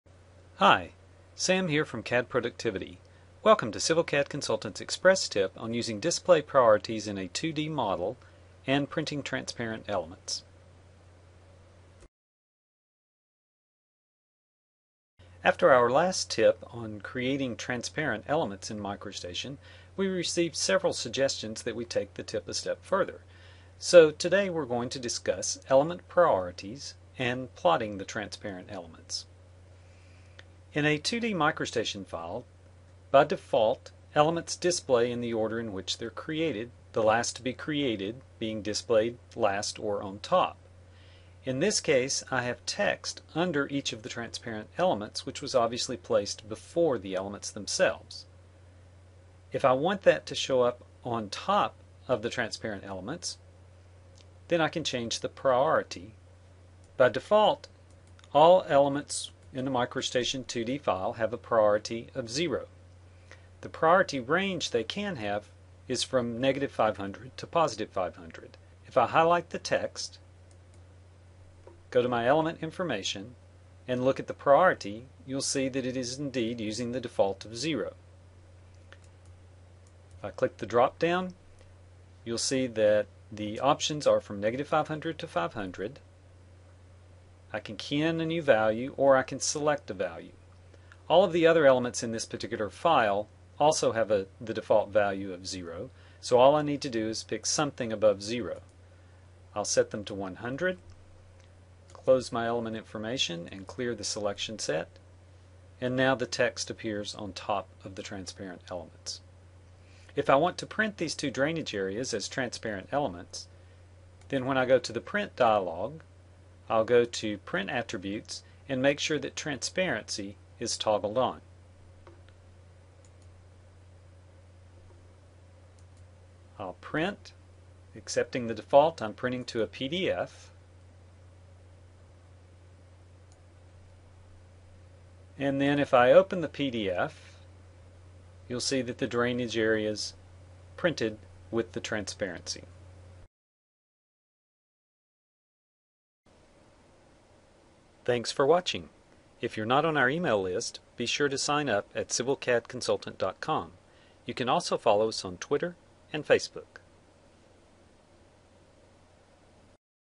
MicroStation Tip: Priorities & Printing Transparent Elements
Setting which MicroStation element displays on top in a 2D model and printing tranparent elements to a PDF. for more InRoads and MicroStation tips. for InRoads training guides and MicroStation training guides.